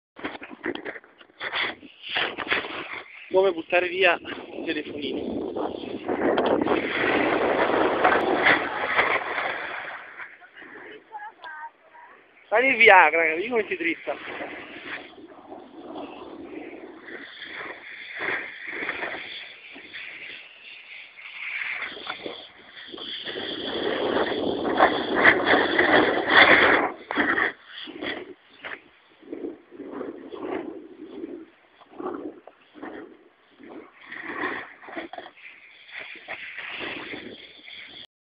SnowBoard
Foppolo Come buttare via un cellulare nuovo di 2 gironi di vita...Quella che inquado è la mia Donna/girl